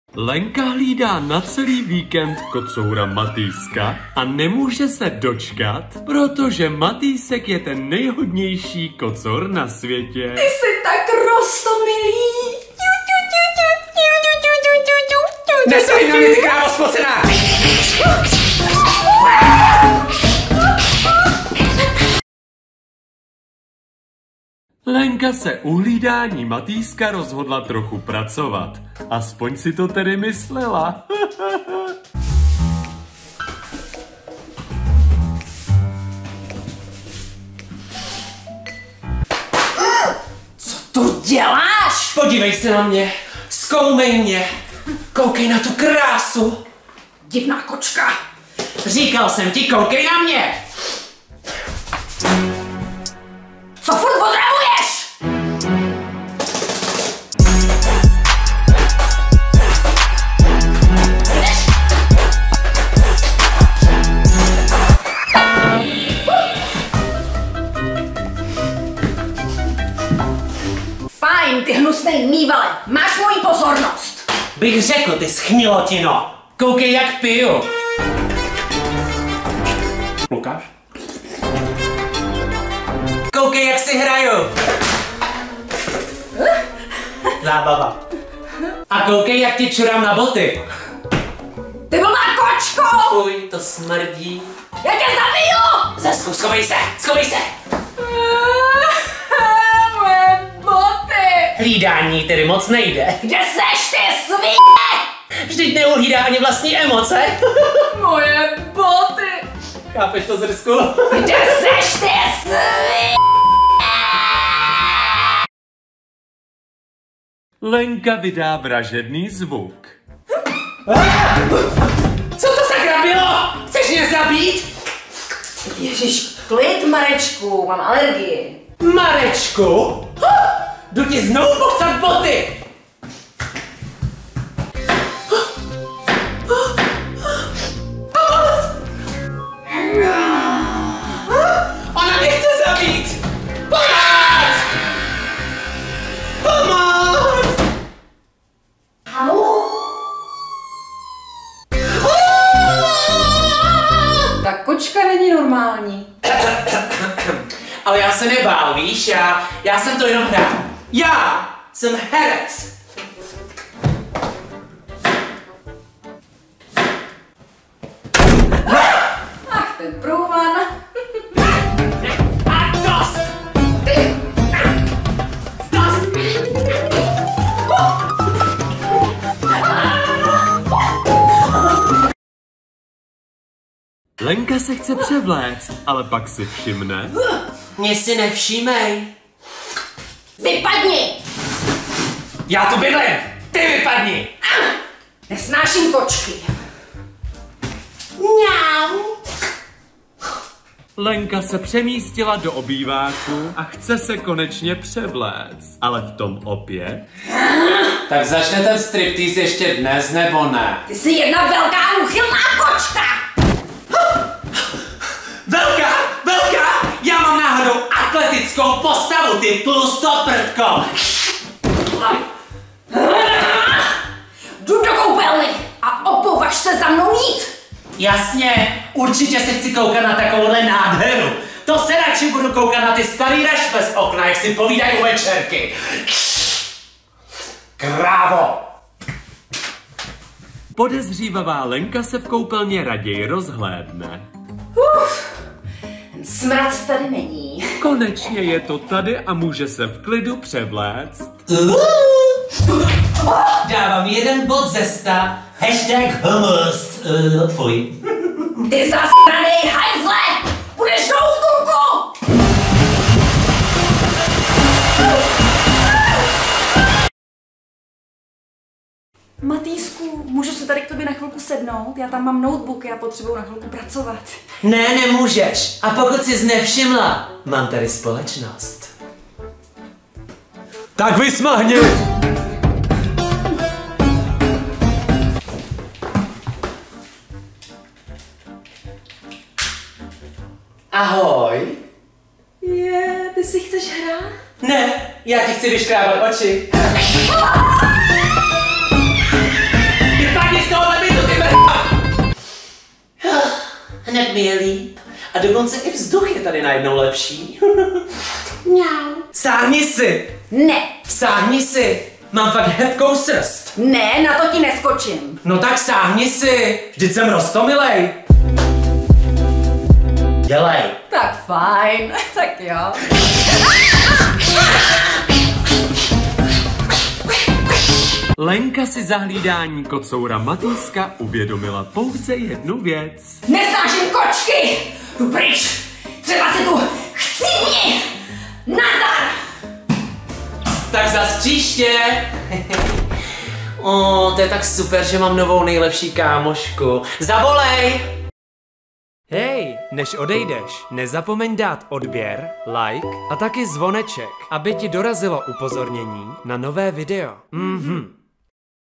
Kdyby kočky mohly mluvit / 5. díl / w. Lenka Breeding
Kocour Matýsek zažije šok! Jeho páníček totiž odjel na celý víkend pryč a to znamená jediné, přijde hlídání v podobě Lenky a to se mu samozřejmě nelíbí. Zvládne Lenka hlídání nejhodnější (hahaha) kocourka na světě nebo skočí z okna? To se dozvíte v dnešním videu.

Podívejte se na další díl – Kdyby kočky mohly mluvit a dejte mi do komentářů vědět, zda vaše číča tohle taky dělá. P.S. Matýsek chce vodu :D